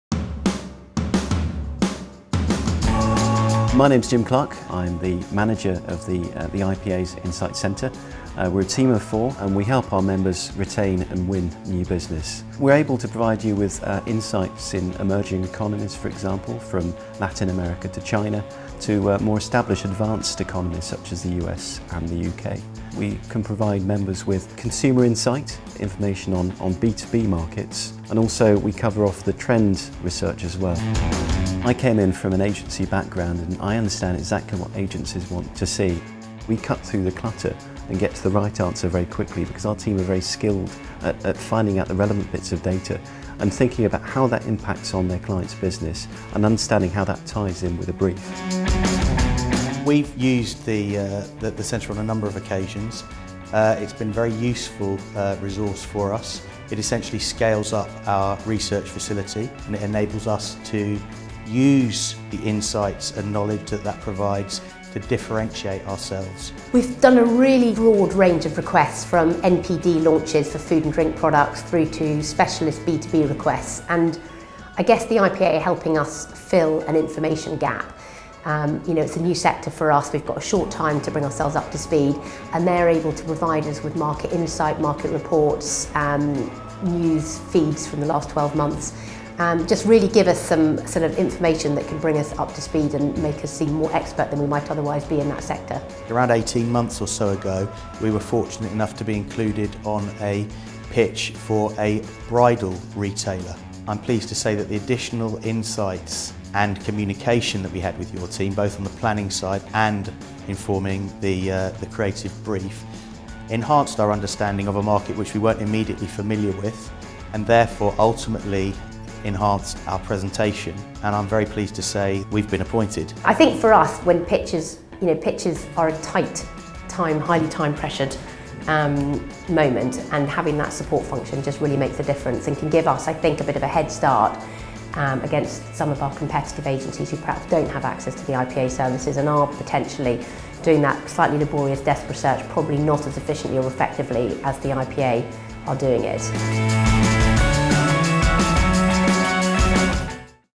What the IPA's insight team can do for you
Find out how the IPA's Insight centre can help you retain and win new business and hear from some agencies we've worked with.